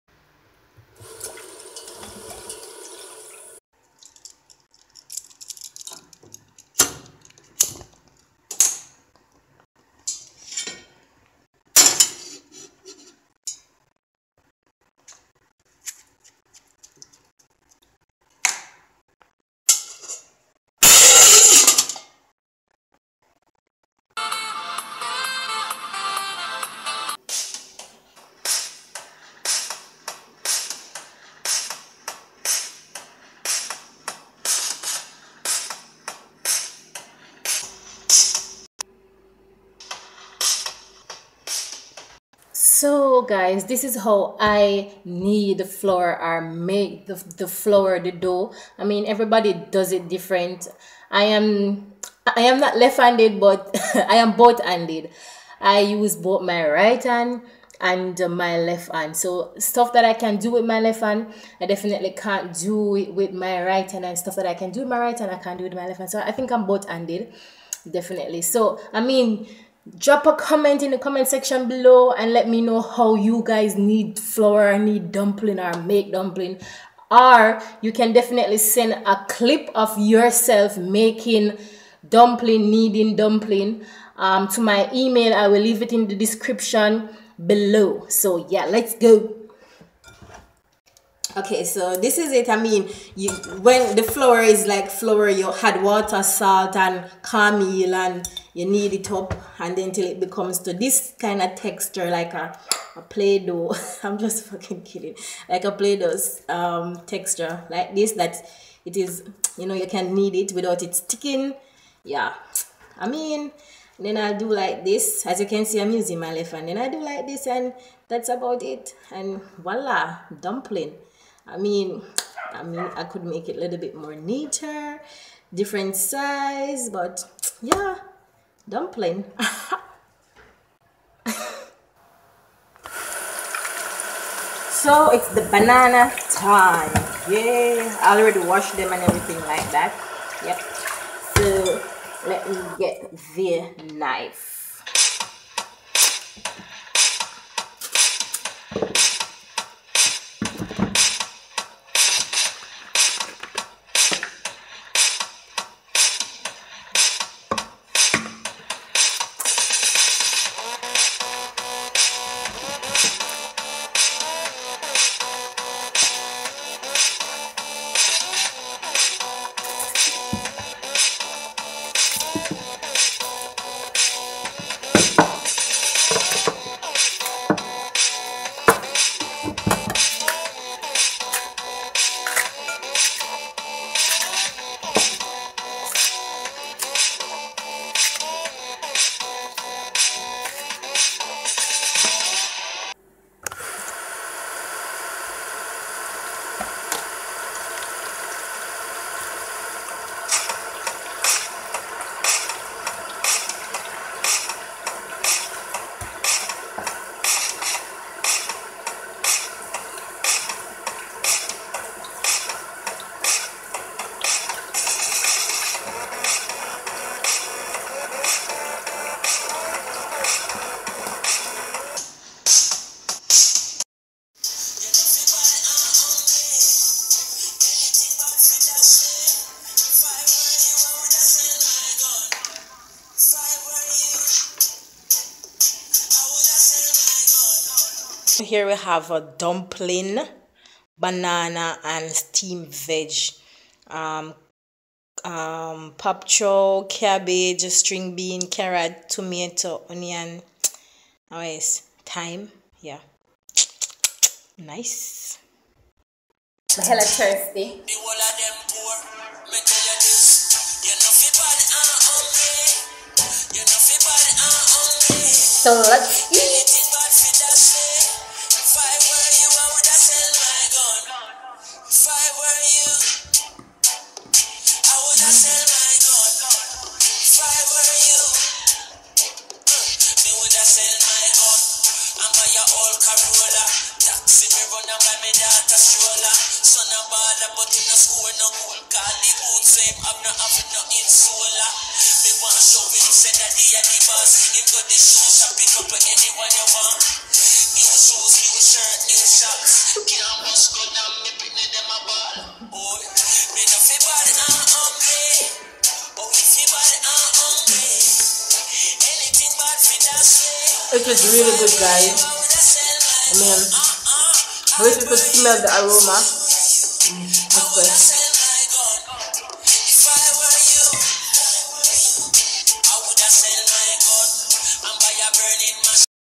Lets cook n eat - nuffi bad n hungry
Hey guys ..so you must be wondering this is a cooking channel one Lol. ..well no it isn't just thought you guys would want to join me for dinner anyways hoped you like